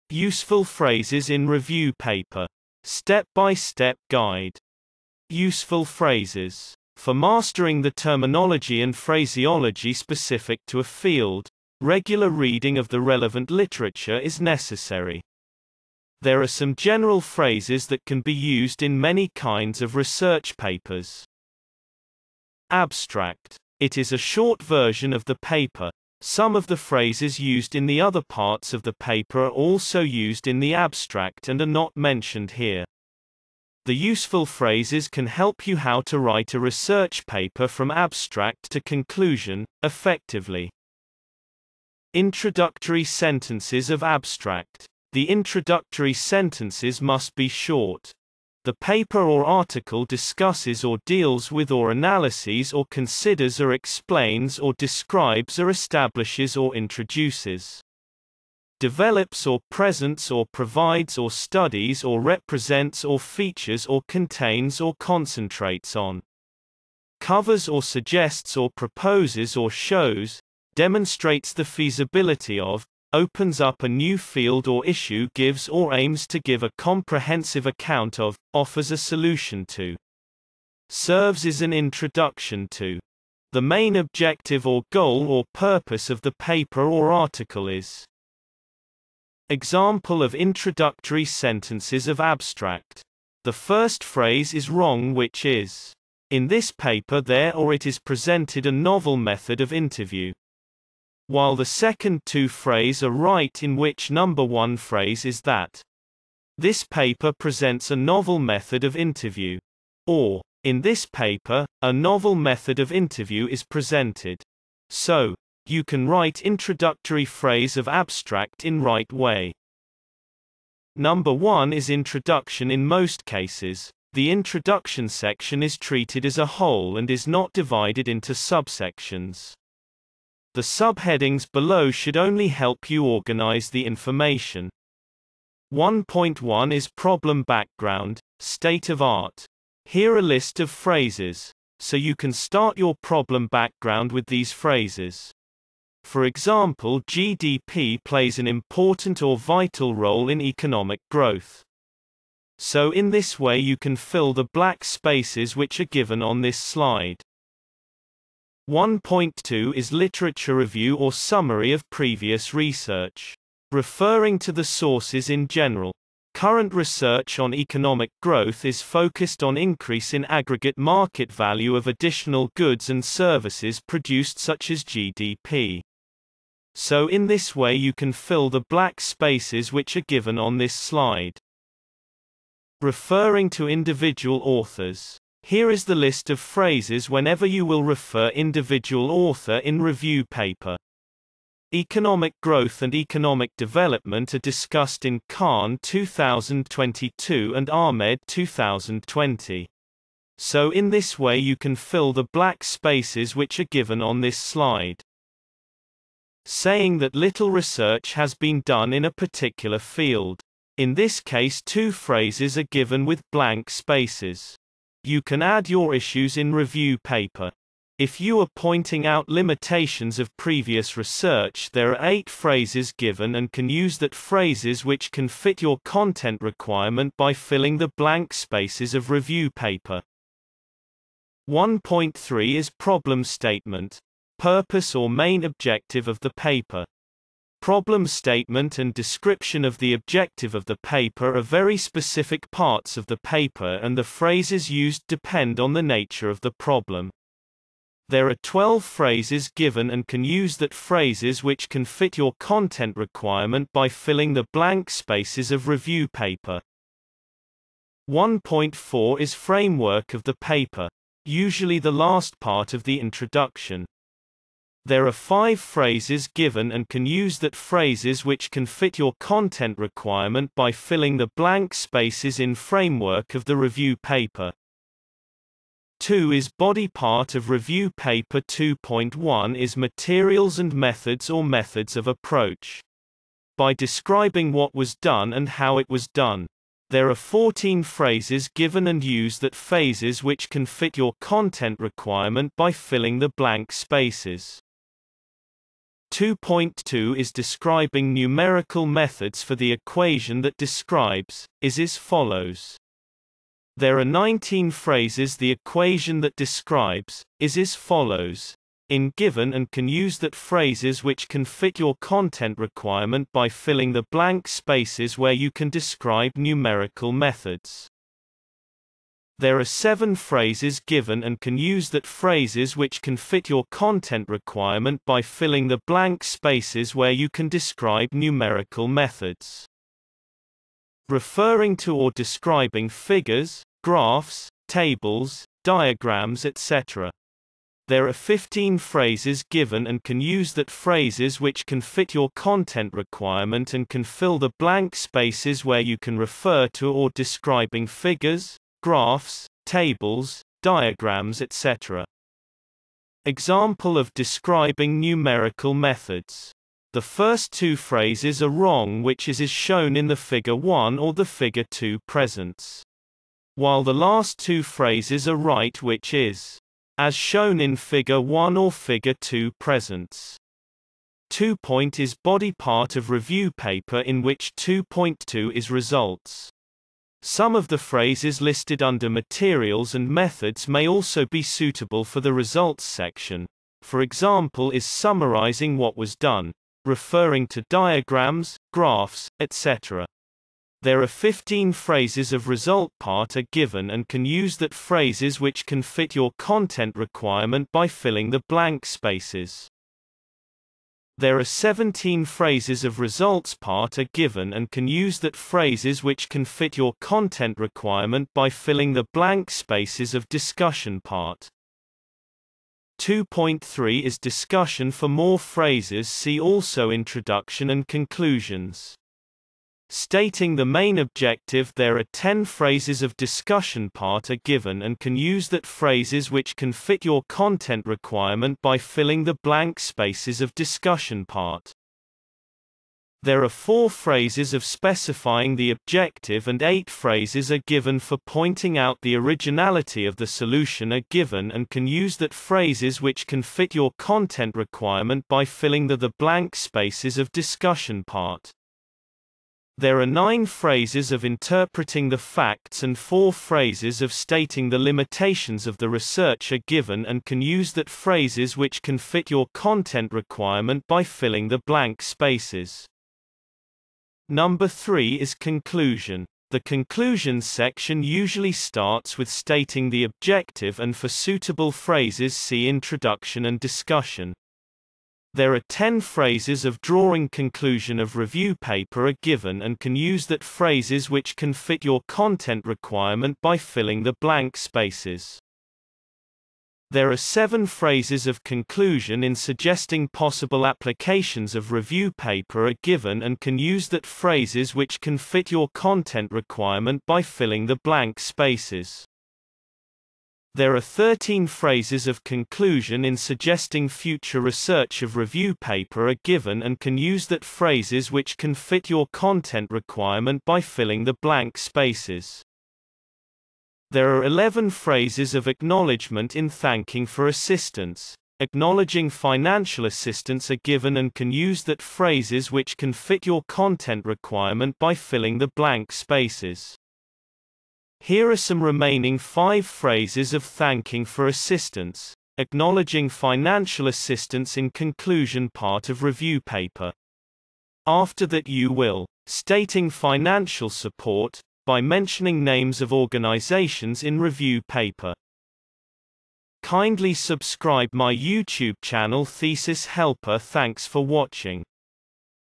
Useful Phrases in Review Paper
This video is about Useful Phrases in Review Paper.
All part of review paper are covered from abstract to acknowledgement.
thesis helper